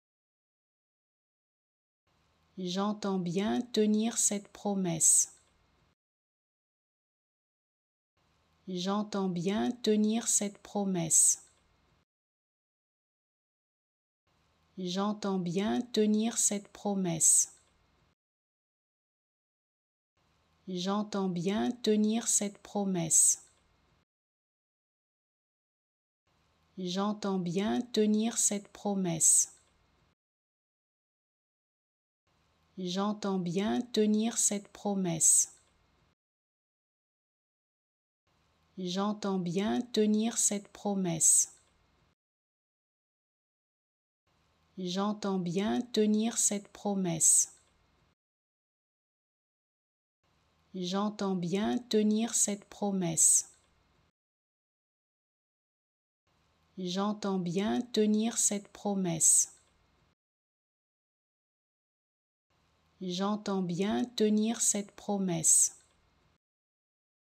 How to Pronounce ''J'entends bien tenir cette promesse'' Correctly in French
Learn how to say and properly pronounce ''J'entends bien tenir cette promesse'' in French with this free pronunciation tutorial. Apprenez à prononcer des mots en français grâce à nos tutoriels.